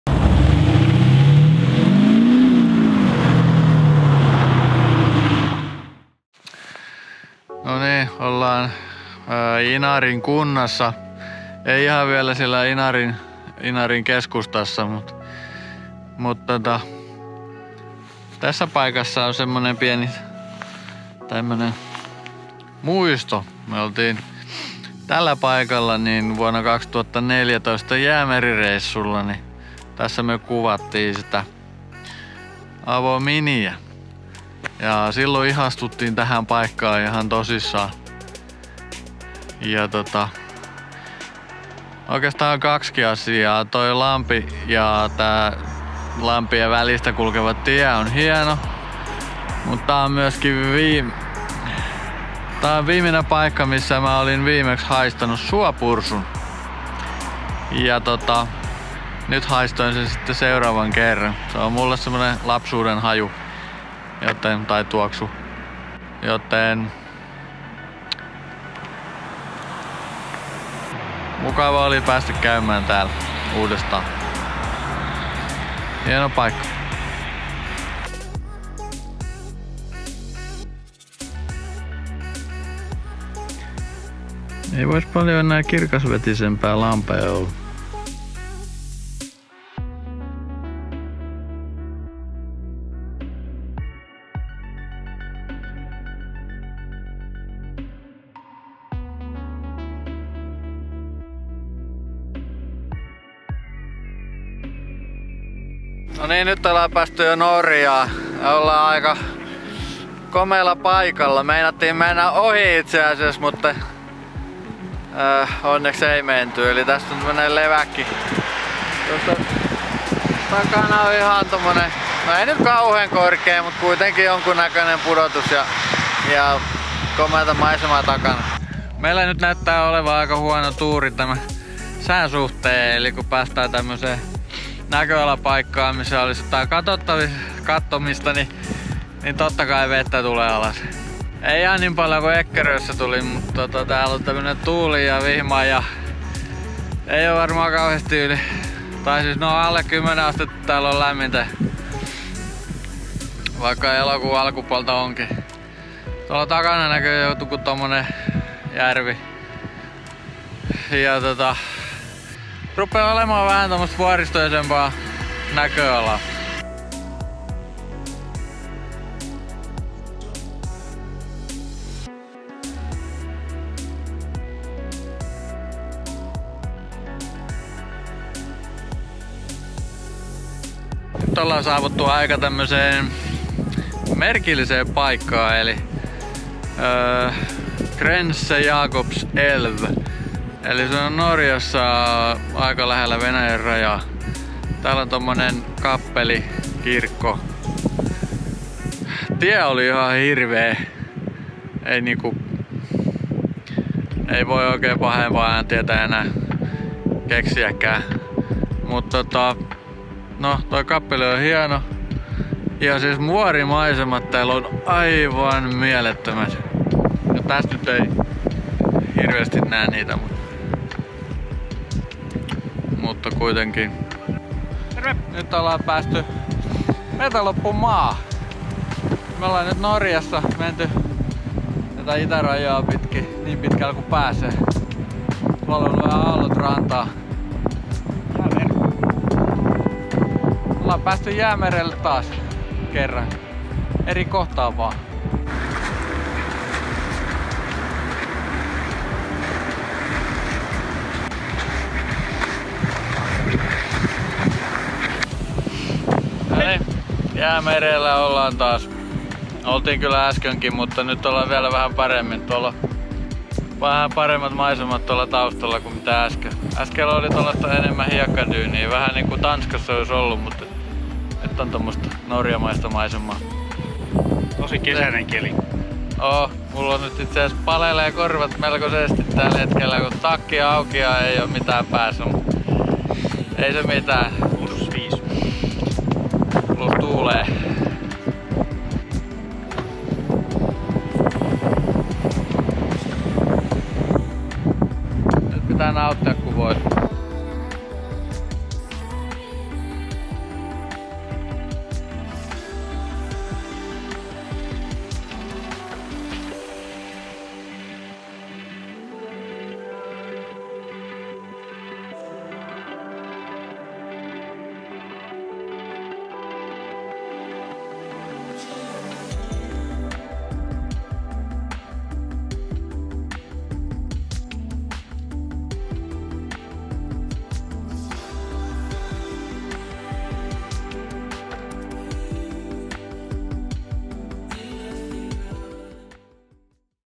Lapland 2019 (Day 2): Saariselkä - Kirkenes
Lapin reissun toinen päivä. Tämän oli ainoa päivä, jolloin oli paljon aikaa ja "vähän" kilometrejä. Silloin tuli myös kuvattua eniten.
Ajoimme Saariselältä Norjaan, kurkkasimme miltä Grense Jacobselv näyttää ja yövyimme Kirkkoniemessä.